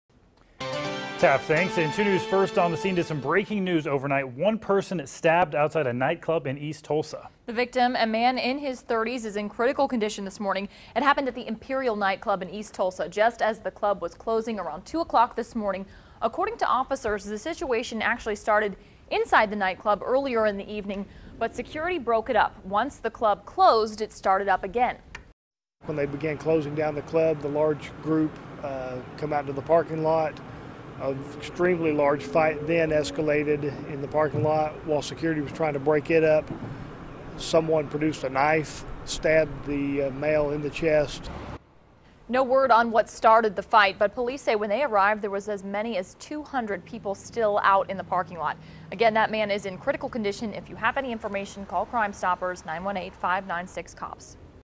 Police: Man stabbed in chest during fight in Club Imperio parking lot
Police are investigating after a man was stabbed outside of an east Tulsa nightclub around 2 a.m. Sunday.